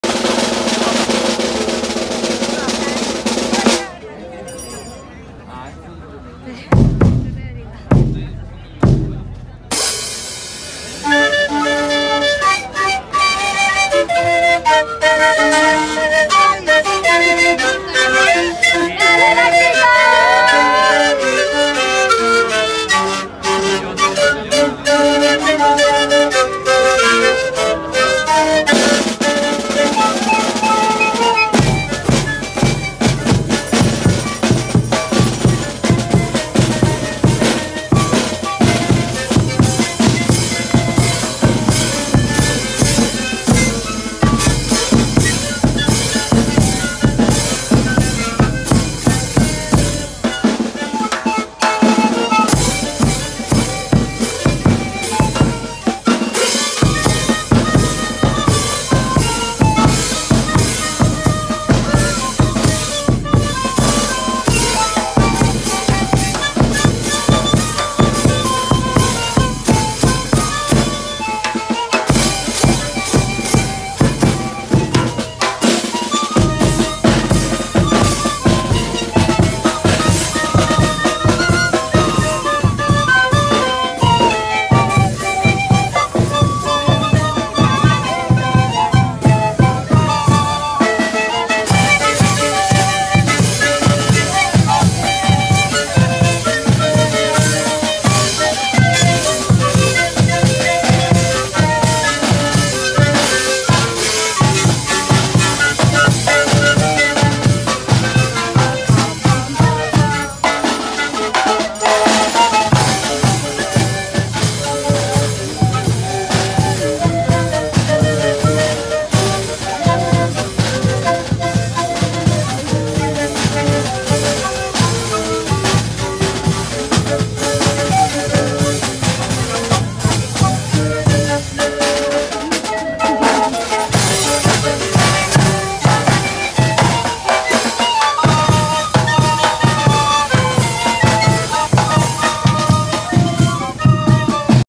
Lakitas Kamanchaka - Mathapi Apthapi Tinku 2023
Lakitas Kamanchaka del Río de la Plata, comparsa oriunda de la Ciudad Autónoma de Buenos Aires, Argentina, y su participación en el 19no encuentro de sikuris “Mathapi Apthapi Tinku”, realizado en el mes de Agosto 2023 en Parque Los Andes, Chacarita, CABA.